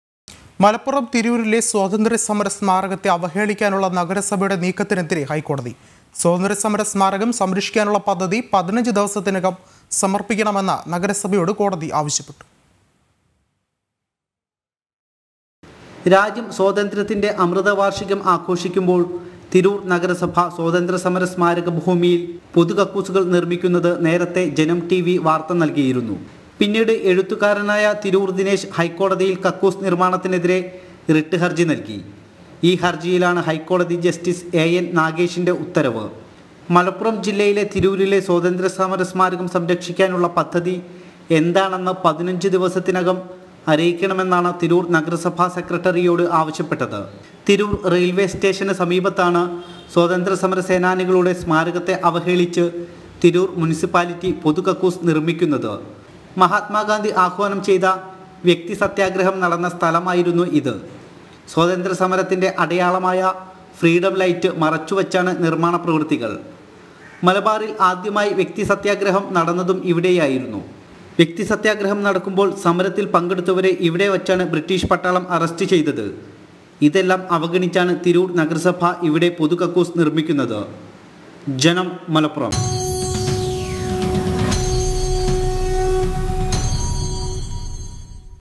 സ്വാതന്ത്ര്യ സമര സ്മാരകത്തെ അവഹേളിക്കാനുള്ള നഗരസഭയുടെ നീക്കത്തിനെതിരെ ഹൈക്കോടതി
മലപ്പുറം തിരൂരിലെ സ്വാതന്ത്ര്യ സമര സ്മാരകത്തെ അവഹേളിക്കാനുള്ള നഗരസഭയുടെ നീക്കത്തിനെതിരെ ഹൈക്കോടതി.